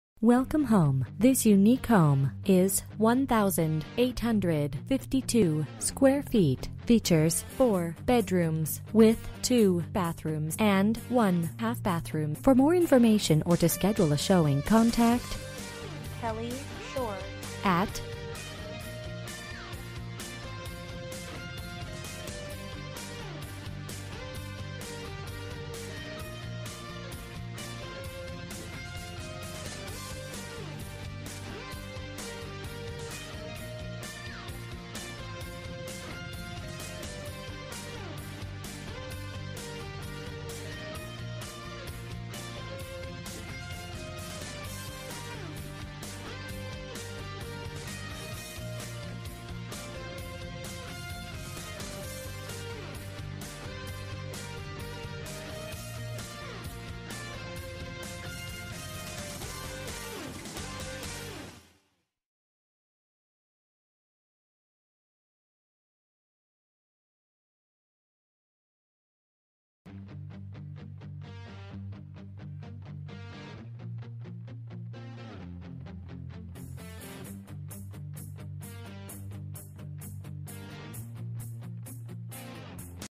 Residential for sale - 2810 S SPENCER AVE W, Magna, UT 84044
Immaculate 4 bd, 2 1/2 bath home w/ remodeled spa-like master bath & walk in closet. Kitchen is updated with fresh paint, counters and flooring. New paint throughout and newer carpet make this home move-in ready. Youll love the wrap around deck & fenced yard but the show stopper is the 40x40 garage w/ car lift, bathroom. compressor, 220 wiring plus a carport!!

Bedrooms: 4
Bathrooms: 2 full and 1 half
Square Feet: 1852
Price: $265,000

For more information about this property, please contact Kelly Shore at  or none. You can also text 5888391 to 67299.

11/09/2019 02:59:07 pm

Last modified: